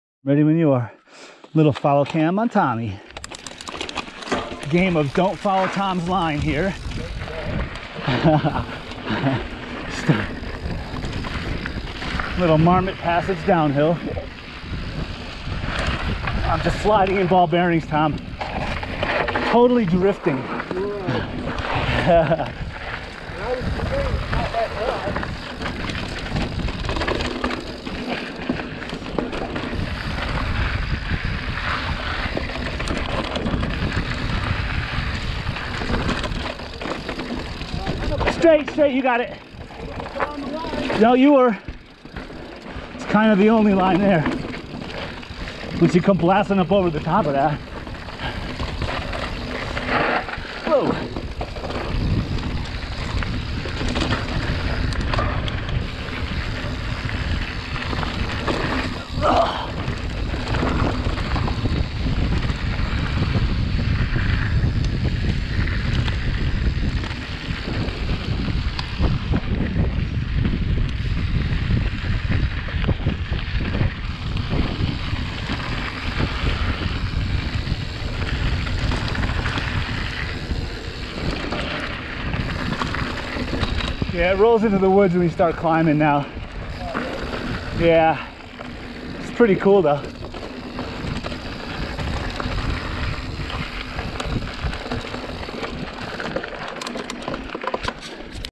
Staunton Park - Marmot Passage - First downhill section
Some follow-cam on Tom on a short downhill section of Marmot Passage Trail headed towards Elk Falls Pond.